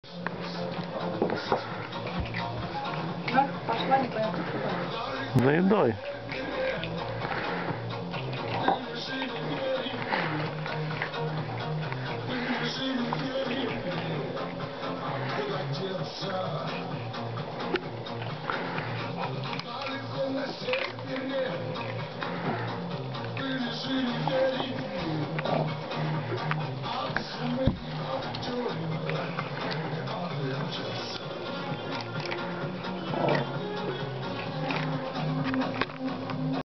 №1, girl Betty ( FOR SALE ), 2 month 5 days old with mother. Pedigree FCI.
kennel Royal Galaxy (FCI)